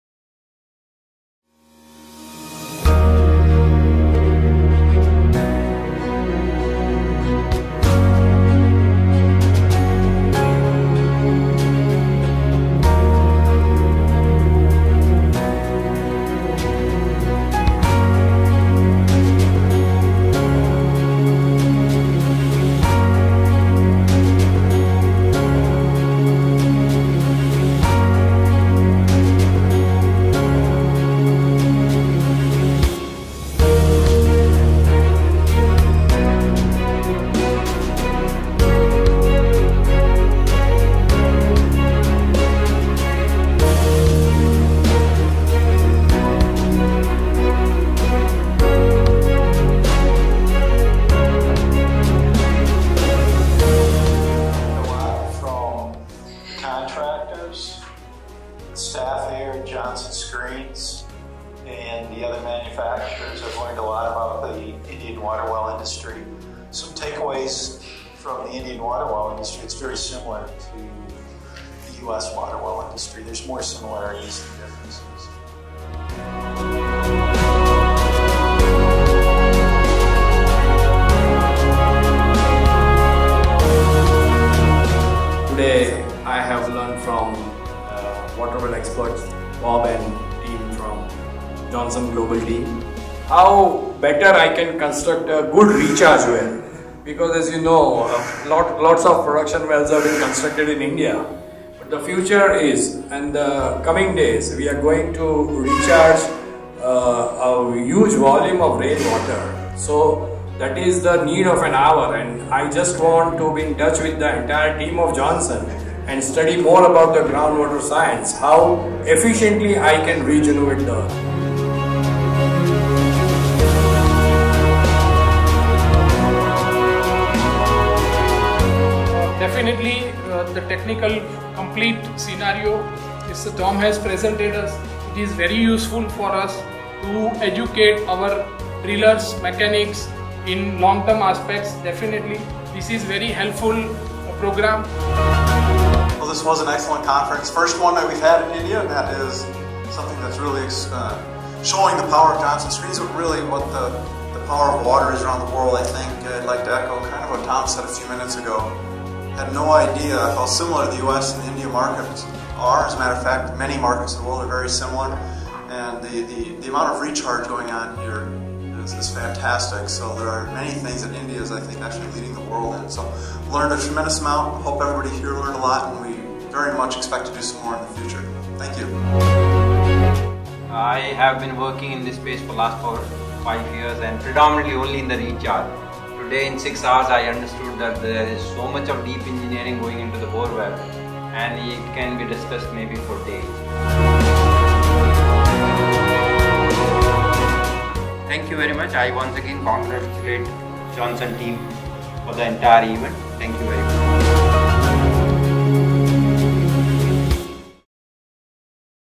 Johnson Screens Groundwater and Wells Class 2022 India
Johnson Screens Expert Thom Hannah recently presented a Groundwater and Wells Class in India.

This was a great opportunity to learn about challenges that face the water wells industry and how similar water wells are across the globe.